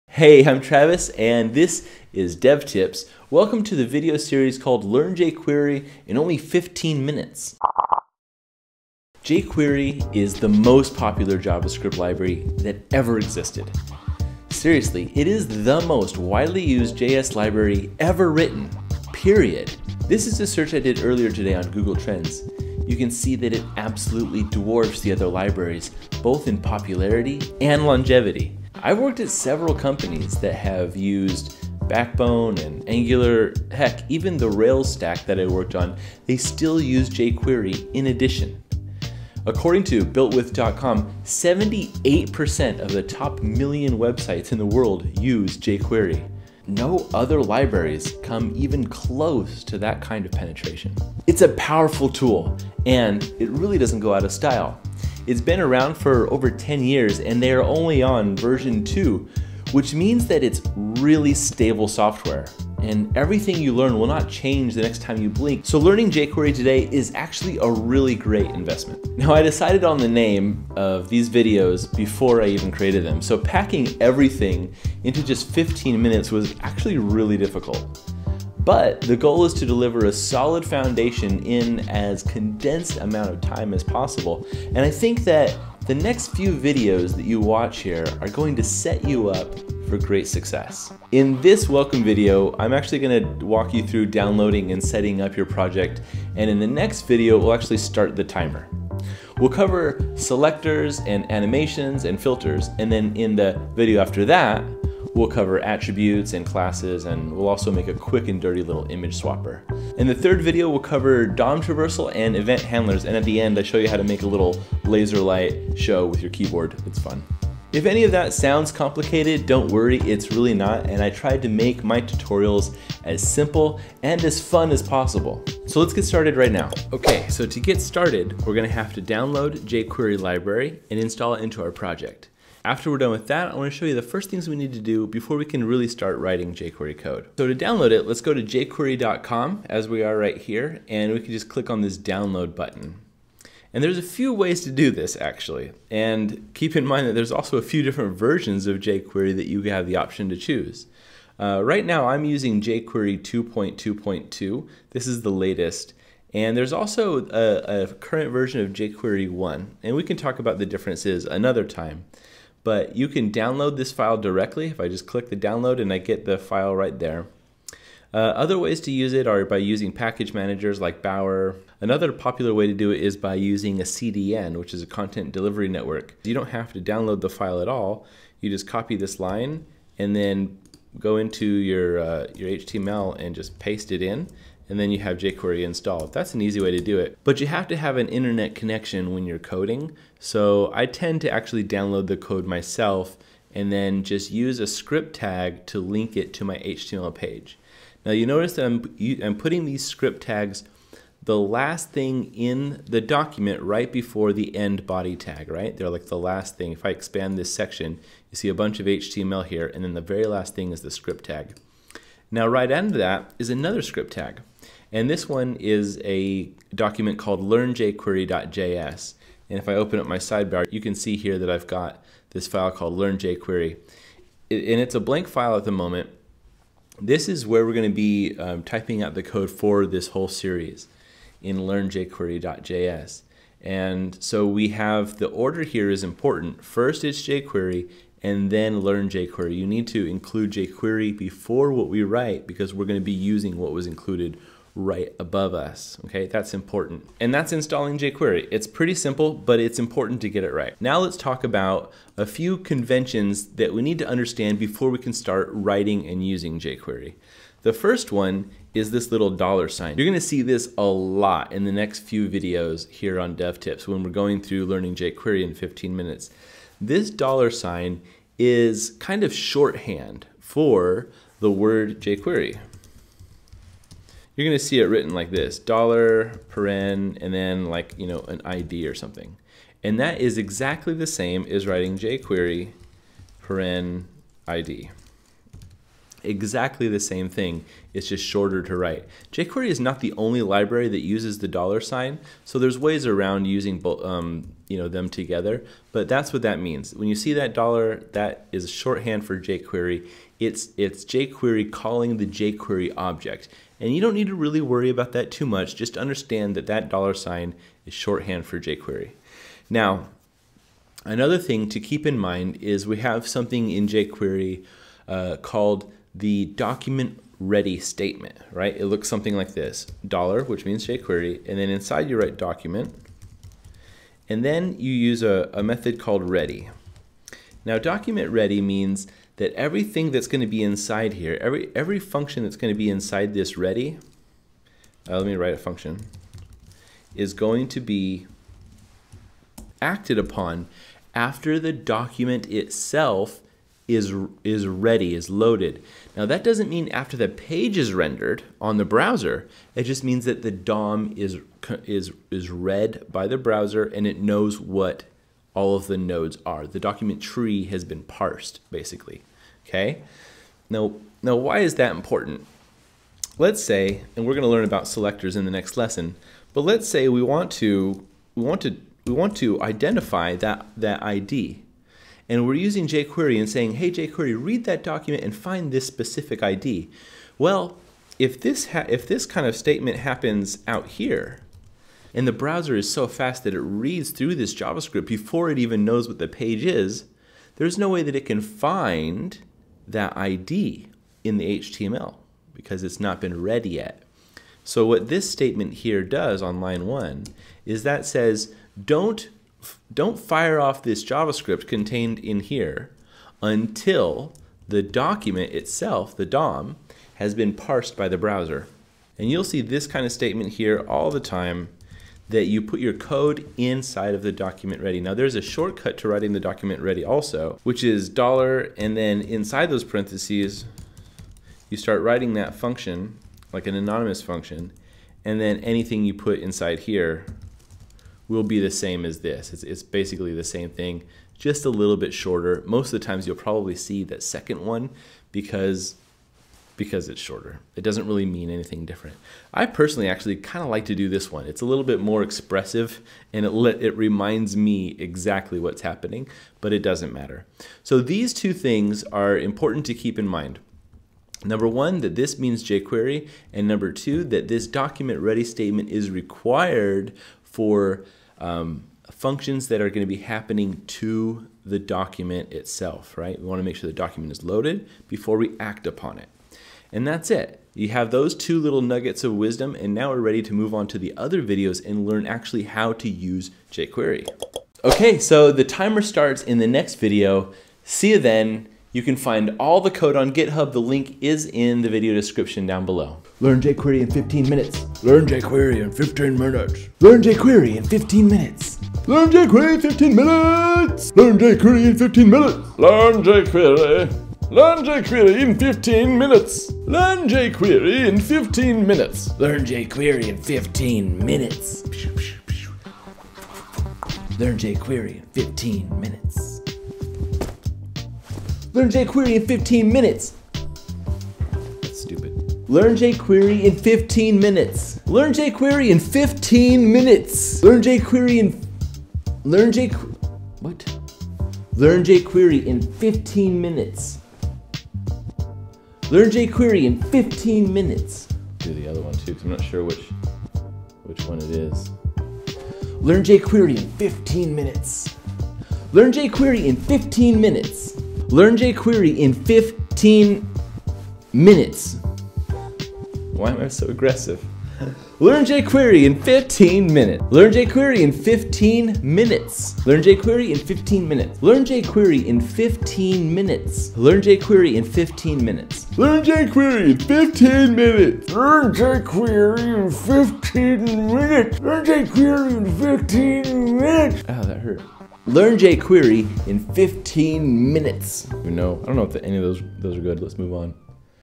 Learn jQuery in 15 minutes – Series Welcome
Welcome to the series called "Learn jQuery in 15 minutes" — In this video we talk about why it's a great idea to know jQuery, and go through an install and setup.